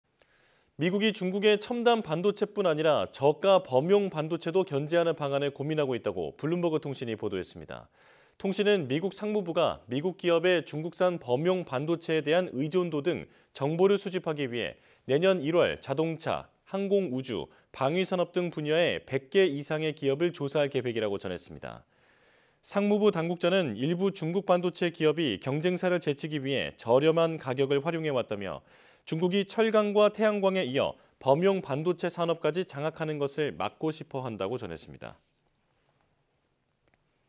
미, 中 첨단반도체에 이어 저가 범용반도체 견제 움직임 / YTN 사이언스
미국이 중국의 첨단 반도체 뿐 아니라 저가 범용 반도체도 견제하는 방안을 고민하고 있다고 블룸버그 통신이 보도했습니다.

통신은 미국 상무부가 미국 기업의 중국산 범용 반도체에 대한 의존도 등 정보를 수집하기 위해 내년 1월 자동차, 항공우주, 방위산업 등 분야의 백 개 이상 기업을 조사할 계획이라고 전했습니다.

상무부 당국자는 일부 중국 반도체 기업이 경쟁사를 제치기 위해 저렴한 가격을 활용해왔다며 중국이 철강과 태양광에 이어 범용 반도체산업까지 장악하는 것을 막고 싶어 한다고 전했습니다.

지나 러몬도 상무장관은 성명에서 지난 몇 년간 중국이 자국 기업의 범용 반도체 생산을 확대하고 미국 기업이 경쟁하는 것을 더 어렵게 만들면서 우려스러운 행동에 나설 가능성이 있다는 징후를 봐왔다며 이번 조사가 우리의 다음 행동에 필요한 정보를 제공할 것이라고 밝혔습니다.

블룸버그는 다음 행동으로 관세나 다른 무역 도구가 포함될 수 있다고 당국자 말을 인용해 전했습니다.

미국 하원의 미중전략경쟁특위는 지난 12일 공개한 보고서에서 중국이 범용 반도체를 지배하지 못하도록 시급히 행동해야 한다며 상무부에 관세 부과를 ...